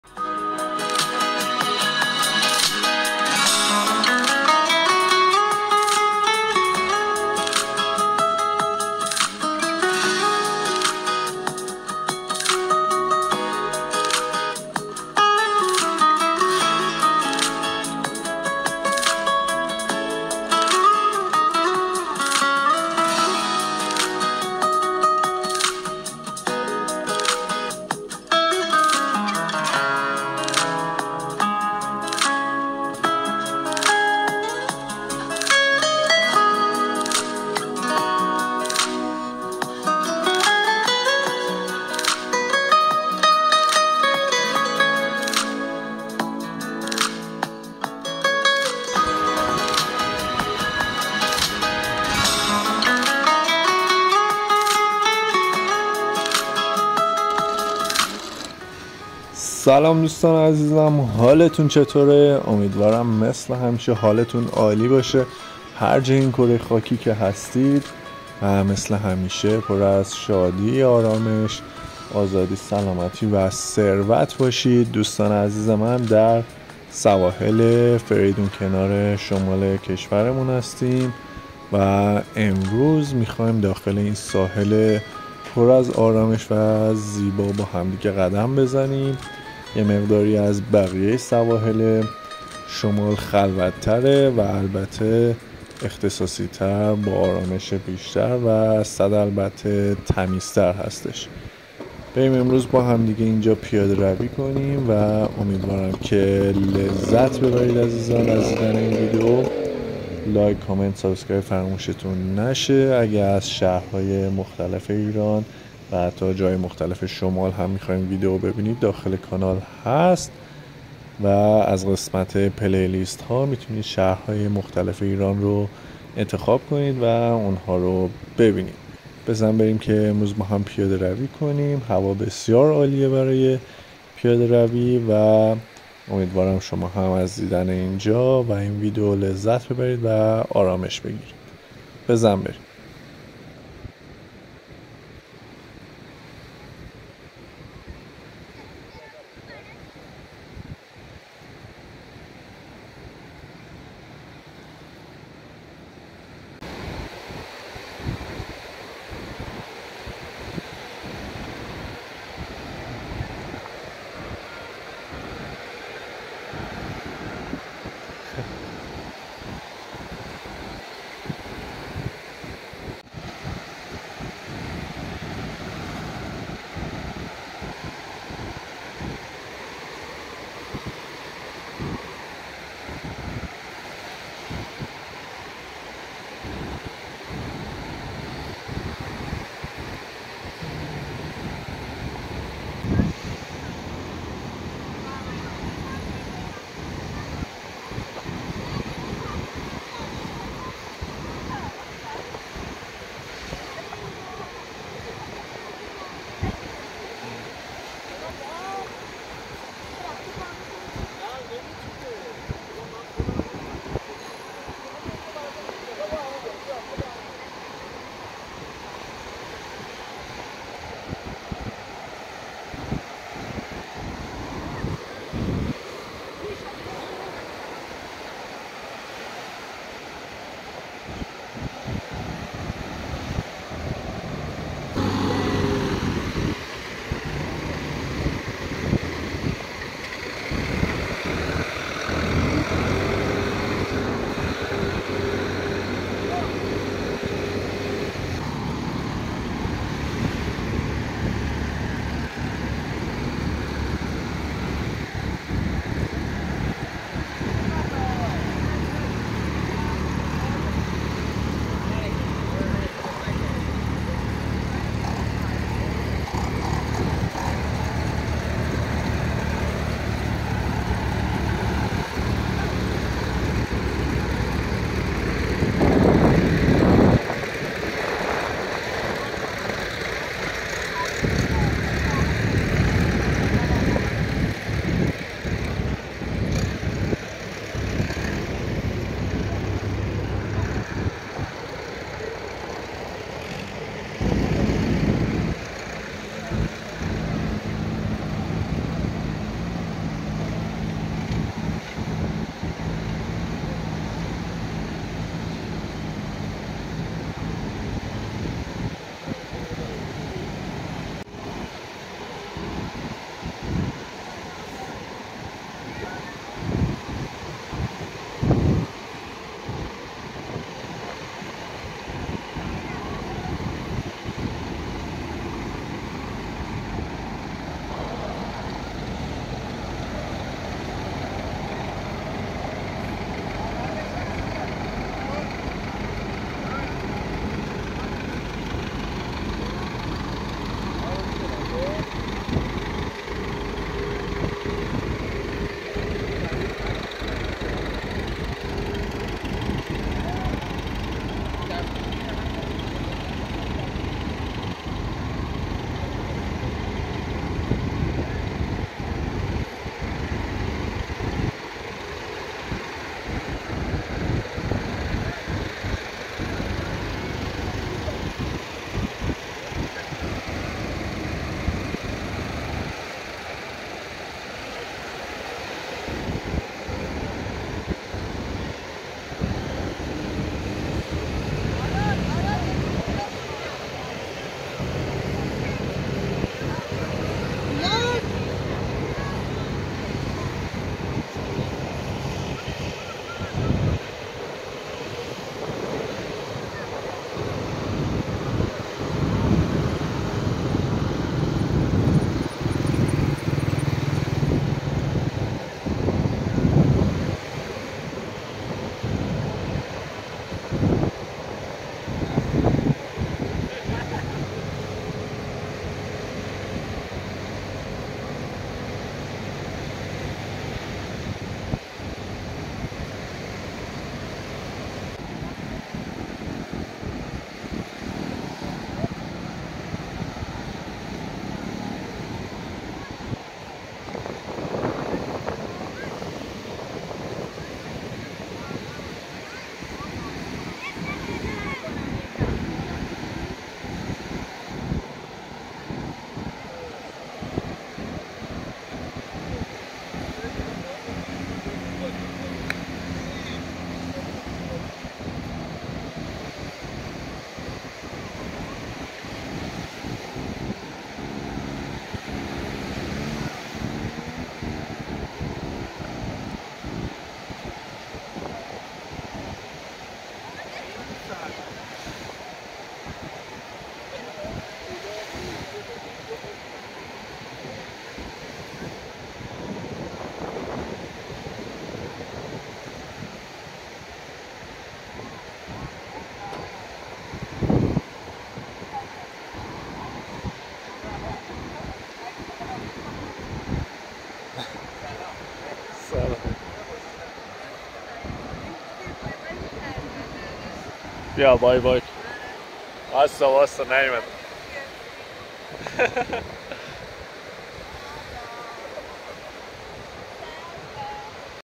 IRAN Today 2023 Vlog. Walk With Me in North Iran. Fereydun kenar Beach. شمال ایران ولاگ 1402.
Walking in IRAN 2023 -